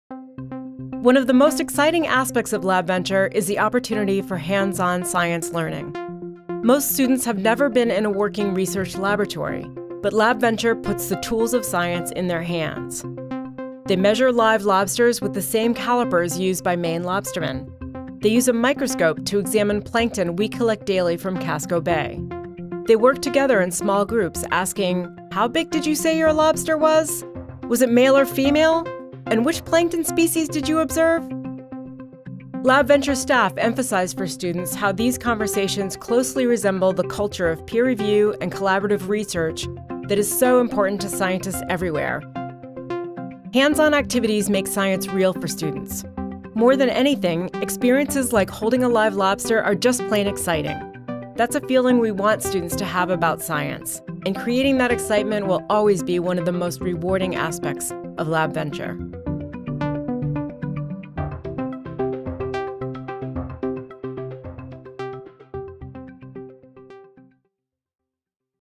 LabVenture: Going Hands-On
Most students have never been in working research laboratory — but LabVenture puts the tools of science in their hands. Watch as they measure live lobsters, examine plankton under a microscope, and collaborate in groups to share their findings.

Hands-on activities make science real for students. More than anything, experiences like holding a live lobster are just plain exciting. That’s a feeling we want students to have about science — and creating that excitement will always be one of the most rewarding aspects of LabVenture.